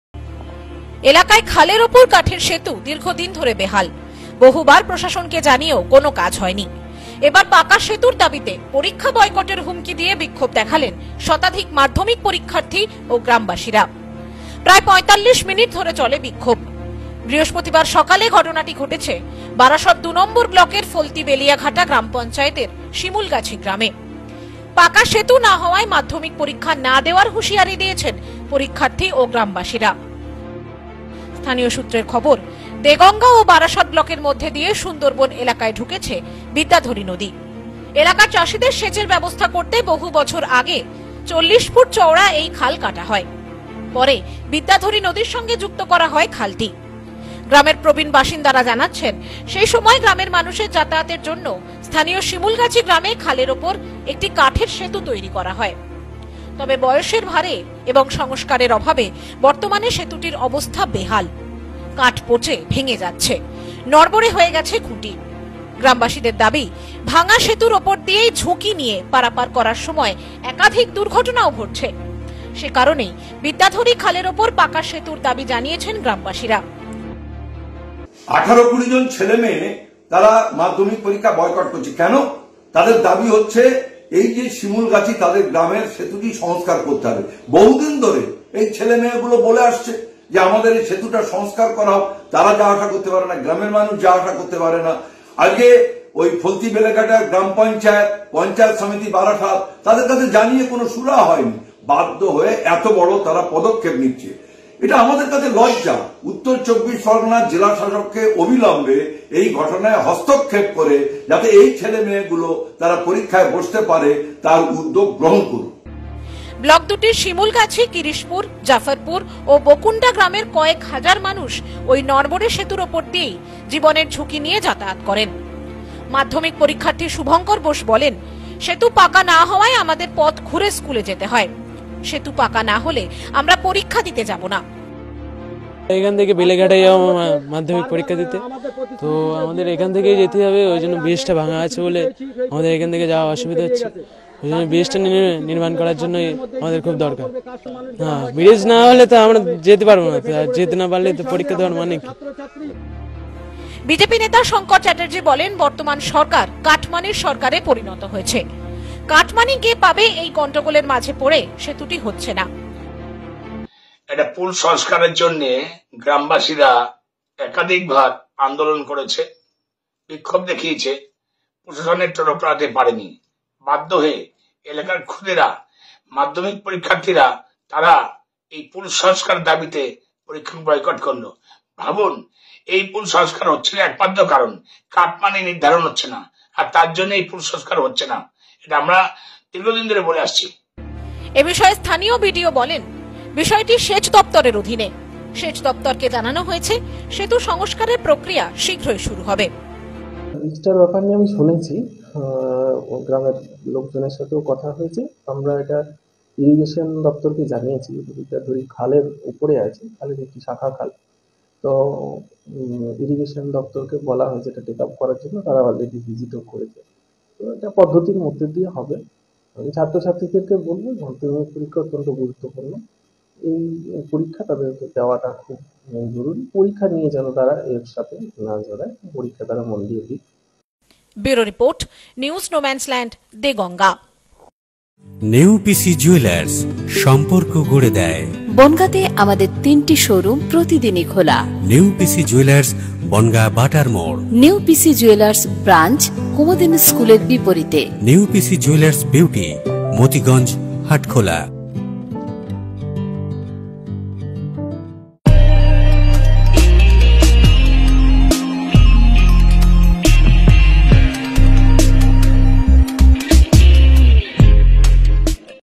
বেহাল সেতু সংস্কারের দাবী জানিয়ে বিক্ষোভ।
বেহাল সেতু সংস্কারের দাবী জানিয়ে বিক্ষোভ। পরীক্ষা বয়কটের হুমকি মাধ্যমিক পরীক্ষার্থীদের।